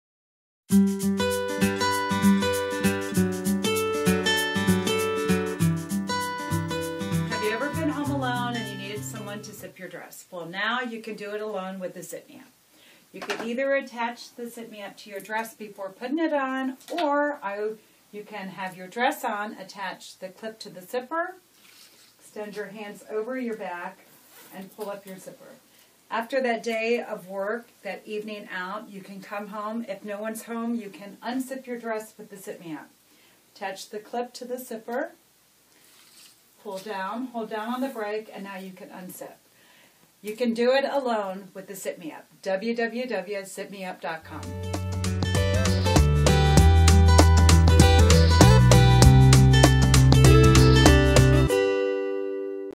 ZipMeUp Inventor Terri Coratola Demonstrates Revolutionary New Product for Women
Terri Coratola, inventor of the ZipMeUp tool for independent women, demonstrates how to use her product.  The ZipMeUp helps a woman to zip or unzip her dress without assistance, and can be used with virtually any zipper type.